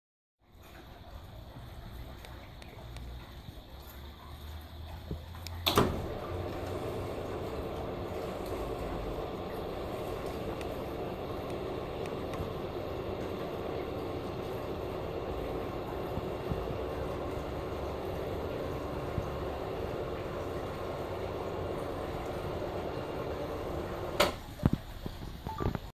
Cable Guide Shades in Plano
Cable guided patio shades in Plano Tx. Blocks wind, sun and gives privacy.